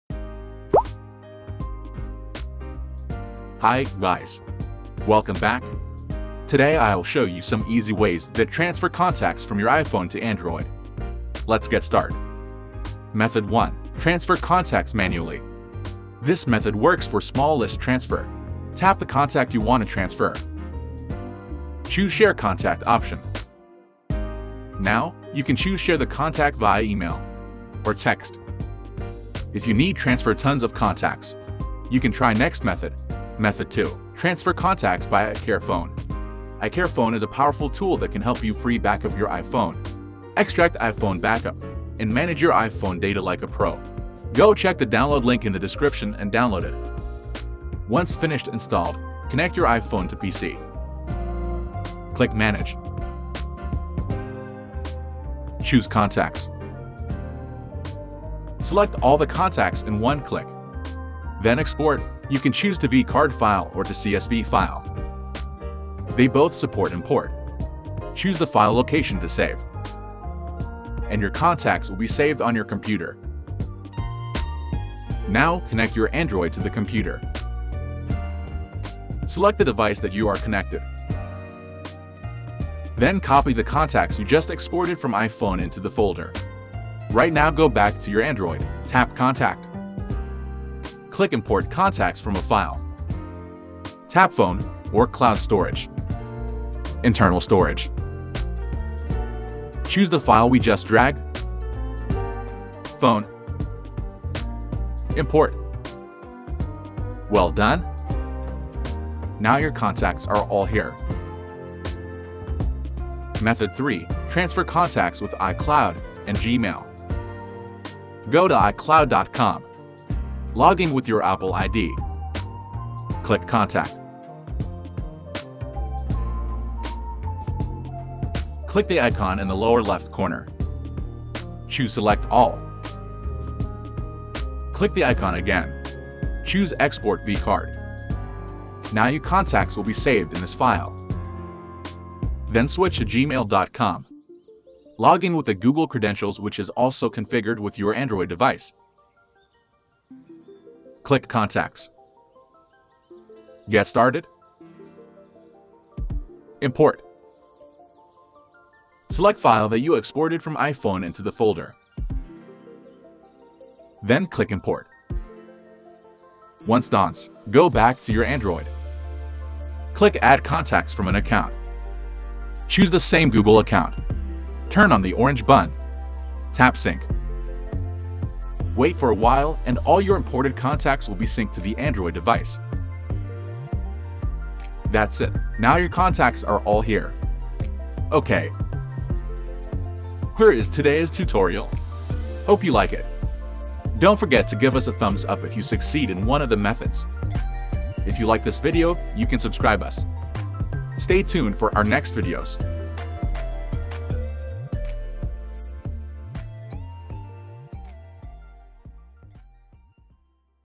How to Transfer Contacts From iPhone To Android [3 Free & Easy Ways]
Hey Guys! Today in this video I will teach you How to Transfer Contacts from iPhone to Android in 2022 with 3 Very easy and Fast Way.

Timestamp:
00:00 Intro & Preview
00:13 Method 1: Transfer Contacts Manually
00:32 Method 2: Transfer Contacts by iCarefone
01:53 Method 3: Transfer Contacts with iCloud and Gmail